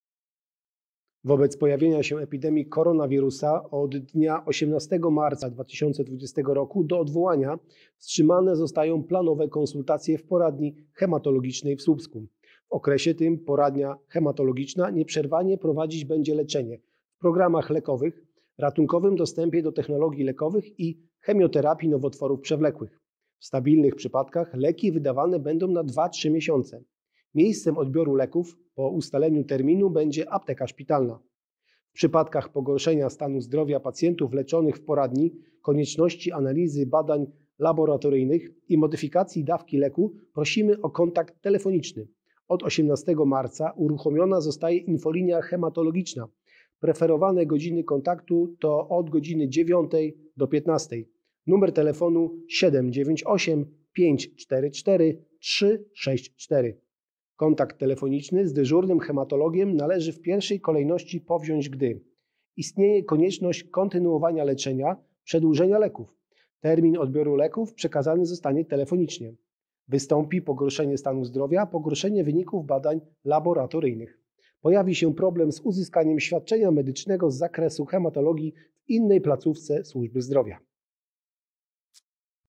Komunikat Poradni Hematologicznej w Słupsku
Wobec pojawienia się epidemii Covid-19 od środy, 18 marca 2020 r., do odwołania wstrzymane zostają planowe konsultacje w Poradni Hematologicznej w Słupsku.

Konsultacje zostają wstrzymane, ale Poradnia Hematologiczna nieprzerwanie prowadzić będzie leczenie w programach lekowych, Ratunkowym Dostępie do Technologii Lekowych i chemioterapii nowotworów przewlekłych. W stabilnych przypadkach leki wydane będą na 2-3 miesiące.

Miejscem odbioru leków, po ustaleniu terminu będzie Apteka Szpitalna.
- W przypadkach pogorszenia stanu zdrowia pacjentów leczonych w poradni, konieczności analizy badań laboratoryjnych i modyfikacji dawki leków prosimy o kontakt telefoniczny - apeluje dr Wojciech Homenda, ordynator oddziału hematologii i transplantacji szpiku.

Od 18 marca 2020 uruchomiona zostaje infolinia hematologiczna.
Numer telefonu : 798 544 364 w godz. 9-15.

Kontakt telefoniczny z dyżurnym hematologiem należy w pierwszej kolejności powziąć, gdy:

a) Istnieje konieczność kontynuowania leczenia , przedłużenia leków. Termin odbioru leków przekazany zostanie telefonicznie.

b) Wystąpi pogorszenie stanu zdrowia, pogorszenie wyników badań laboratoryjnych.

c) Pojawi się problem z uzyskaniem świadczenia medycznego z zakresu hematologii w innej placówce służby zdrowia.